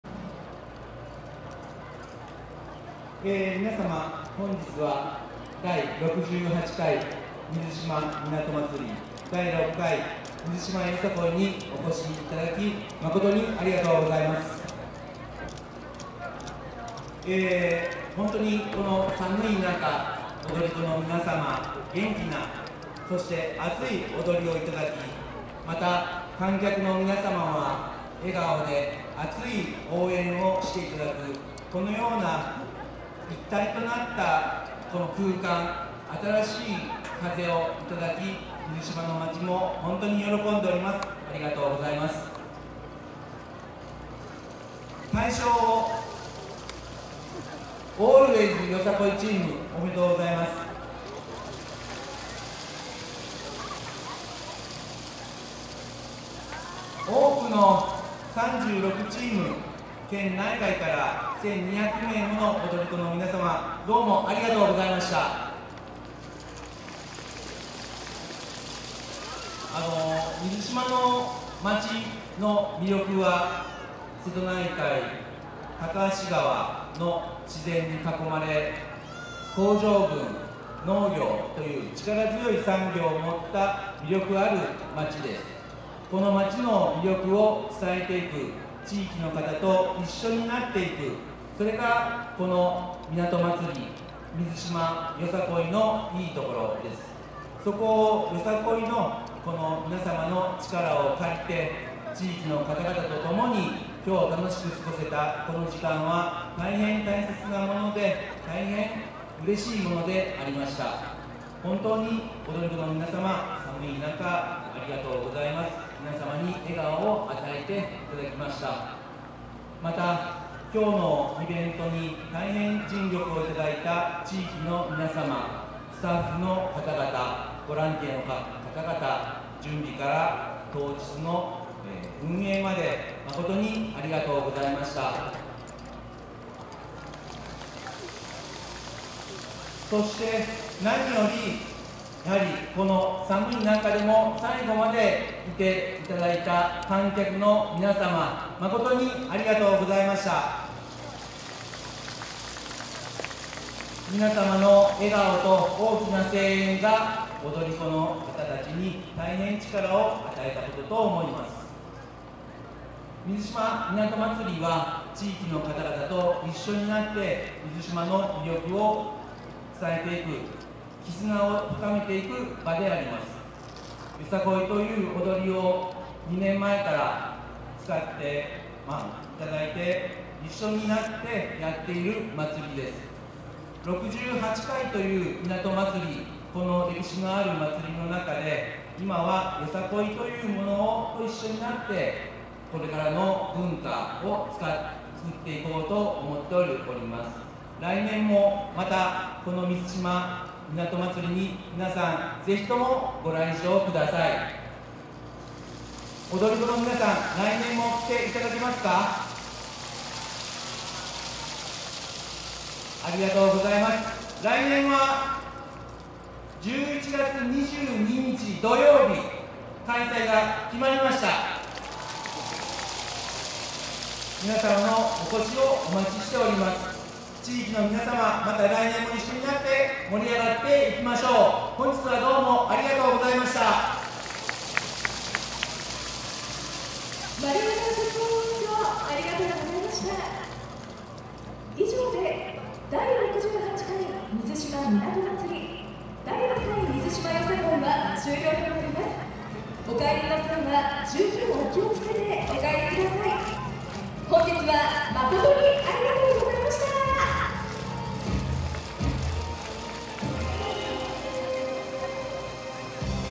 閉会式 📍第６回 水島よさこい (よさこい大通り) 2024-11-23T18:55
2024年11月23日(土)、岡山県倉敷市で開催された「第６回  水島よさこい / 第68回 水島港まつり」(よさこい大通り)での「閉会式」です。